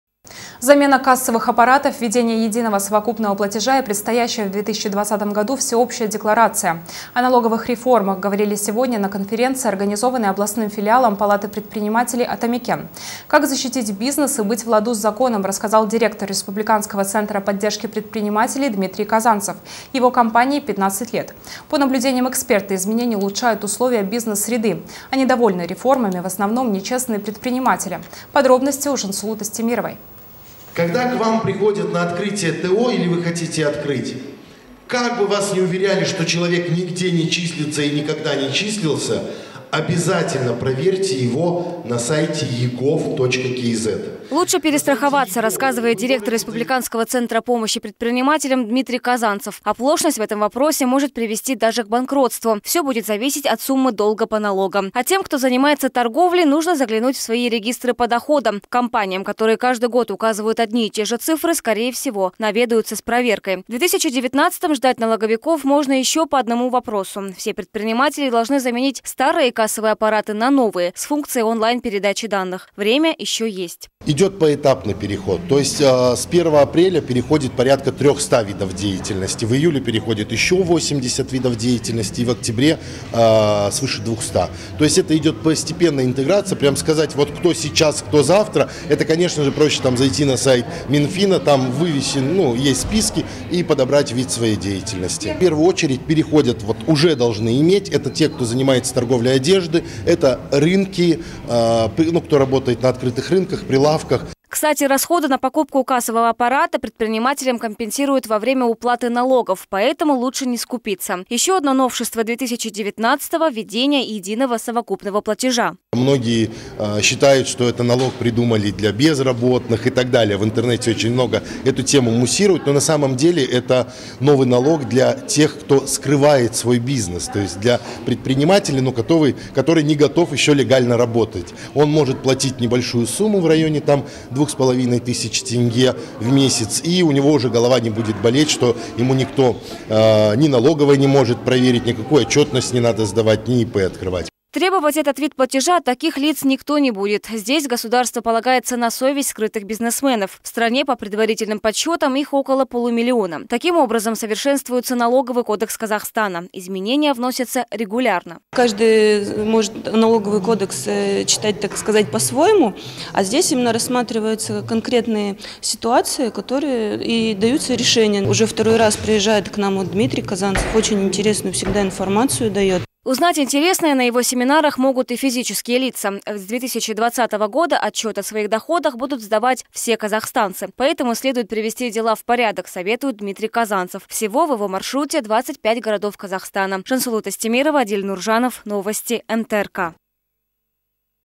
О налоговых реформах говорили сегодня на конференции в палате «Атамекен»
Замена кассовых аппаратов, введение единого совокупного платежа и предстоящая в 2020 году всеобщая декларация. О налоговых реформах говорили сегодня на конференции, организованной областным филиалом Палаты предпринимателей "Атамекен". Как защитить бизнес и быть в ладу с законом, рассказал директор республиканского центра поддержки предпринимателей Дмитрий Казанцев. Его компании 15 лет. По наблюдениям эксперта, изменения улучшают условия бизнес-среды, а недовольны реформами в основном "нечестные" предприниматели. Подробности далее.